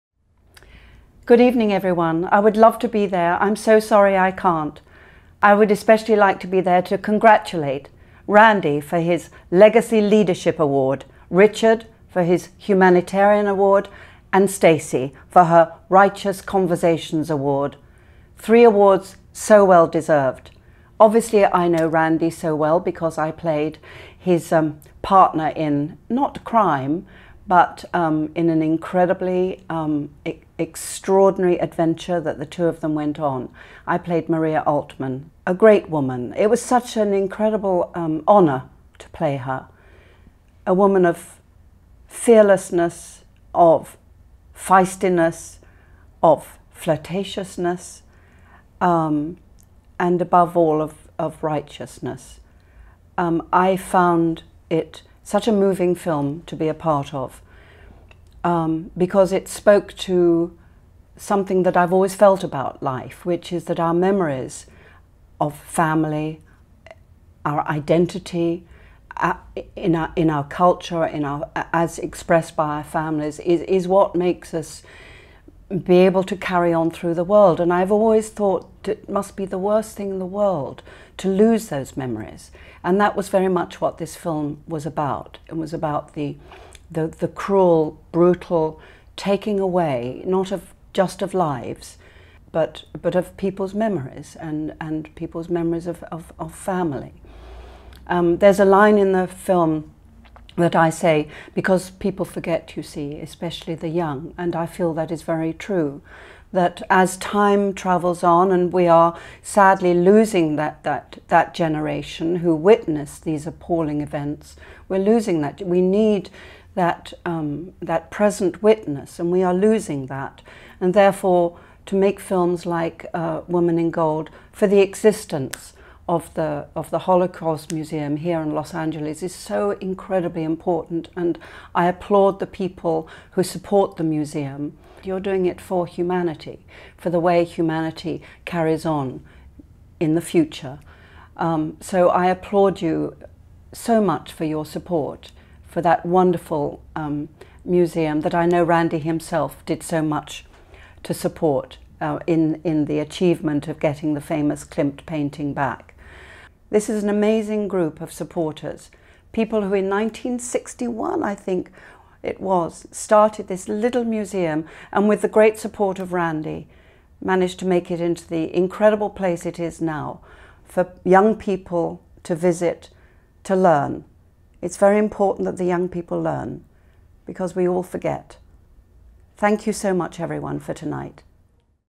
Helen Mirren for the LAMOTH 2015 Annual Gala
Helen Mirren gives her thoughts on the film Woman in Gold as part of a video for the 2015 Annual Gala for Los Angeles Museum of the Holocaust honoring Randy Schoenberg and Richard Jones.